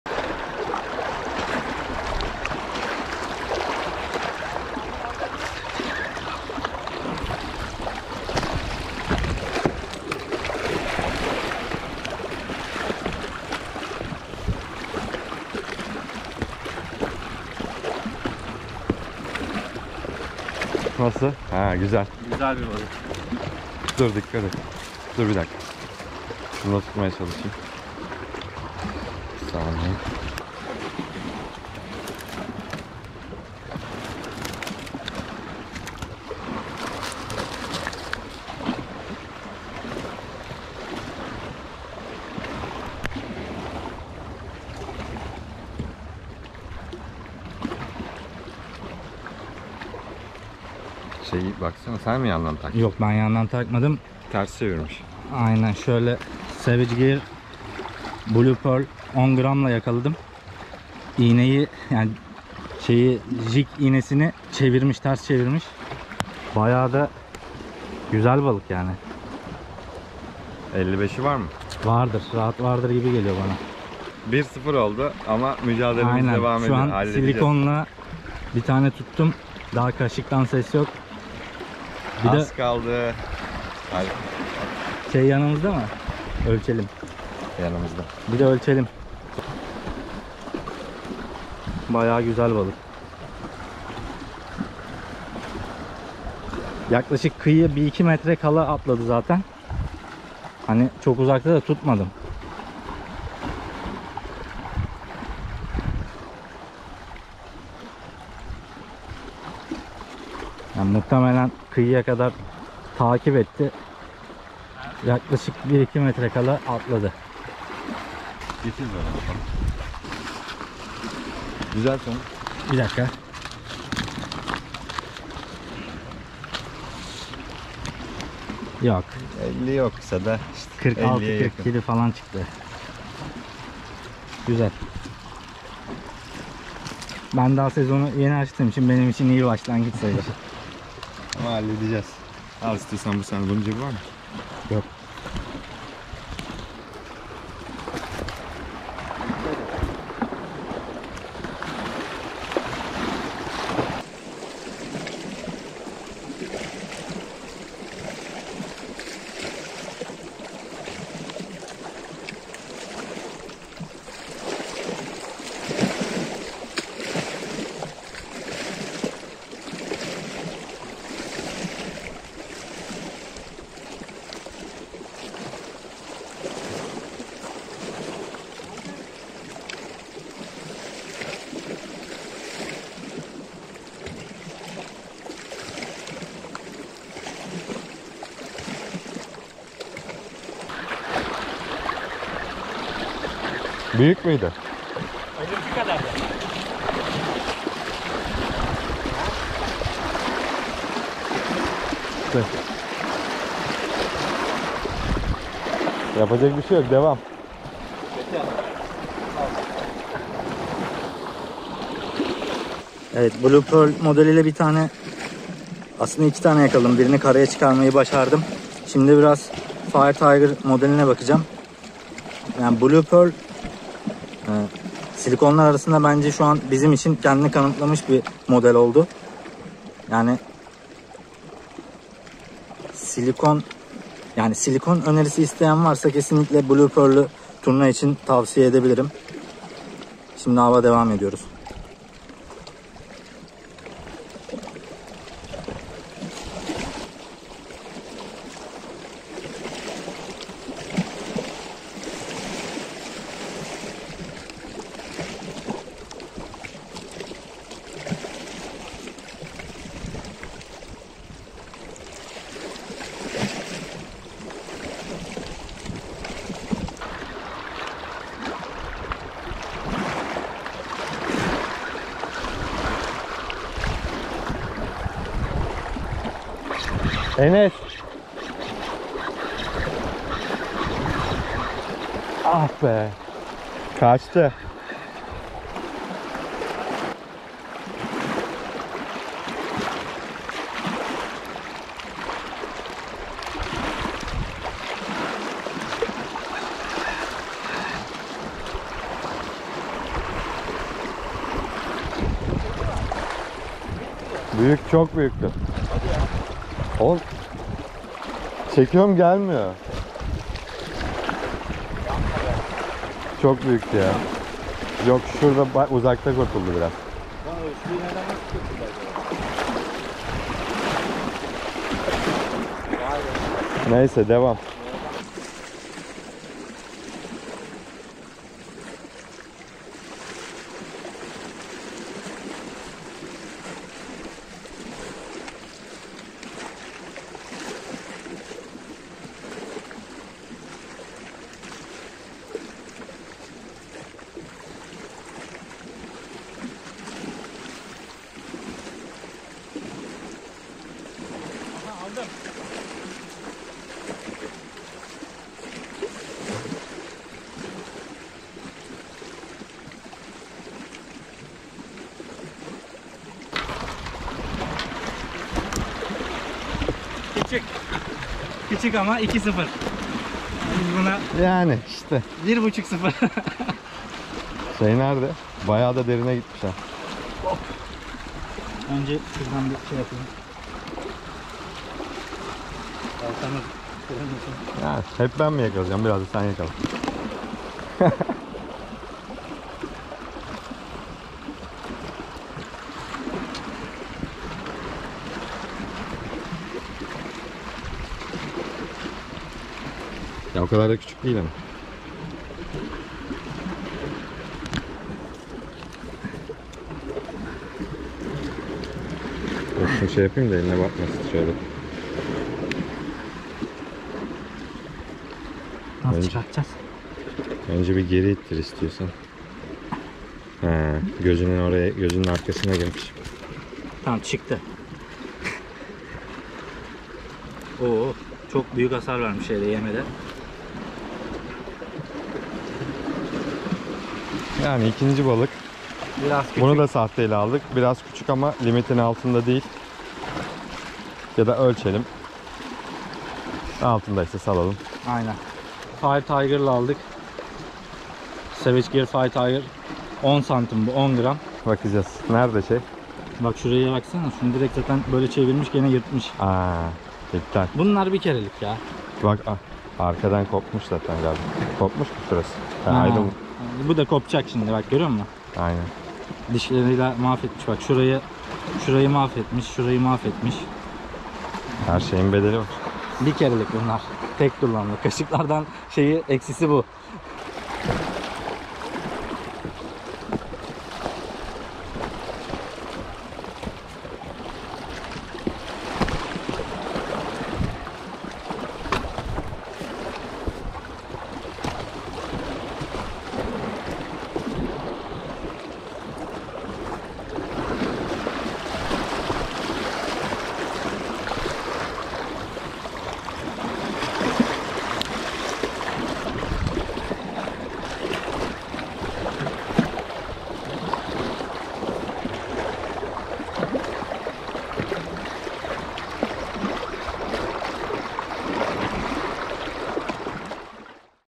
Kıyıdan Silikon Yem ile Turna Avı
Turna avı maceralarımız hız kesmeden devam ediyor. Bu defa göl kenarından yaptığımız av sırasında silikon yemler ile güzel balıklar aldık.

Sabah erken saatlerde kıyıdan at çek ile turna avı için denemelerimize başladık. Avın başında kaşık ve Savage Gear Cannibal Shad marka silikon yemleri tercih ettik. Ava henüz yeni başlamıştık ki Blue Pearl (10cm 9gr) modeli ile güzel bir turna yakaladık.

At çek yaparken Blue Pearl modeli sadece silikon yem çeşitleri içinde değil, genel anlamda şu ana dek en çok verim aldığımız model oldu diyebiliriz. Sırt kısmındaki mavi detayları suyun altında maksimum görünürlük sağladığı için kapalı havalar veya bulanık sularda dahi iyi sonuçlar almanızı sağlayan bir yem.

Avın devamında Blue Pearl ile bir tane daha yakalamış olmamıza rağmen kepçemizi unuttuğumuz için balığı kenara alamadım. Bir tane de kaşık ile yakaladığımız oldukça büyük boyutlardaki bir turnayı yine kenara almayı başaramadık.

Daha sonra Cannibal Shad Fire Tiger (10cm 9gr) modeli ile ava devam etmeyi tercih ettim ve ikinci turna balığını da çok geçmeden kıyıya almış olduk.

Turna avı devamında silikon yemler ve kaşıklar ile denemelerimiz sürse de oltamıza başka balık takılmadı ve balık avı son buldu.

Herkese iyi seyirler, rastgele.